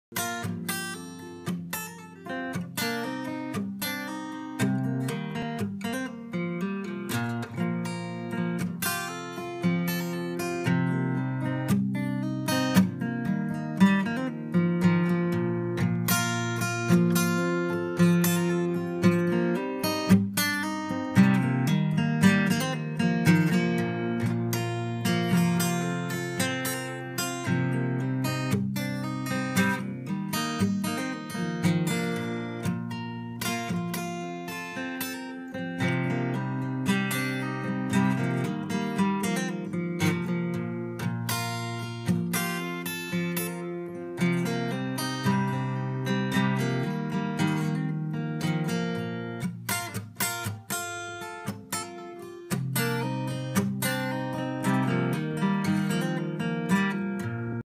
Ceol Waves Premium Zinc Alloy Capo and Medium Gauge Celluloid Picks
Zach G playing the guitar using his Ceol Waves Premium Zinc Alloy Capo and Medium Gauge Celluloid Picks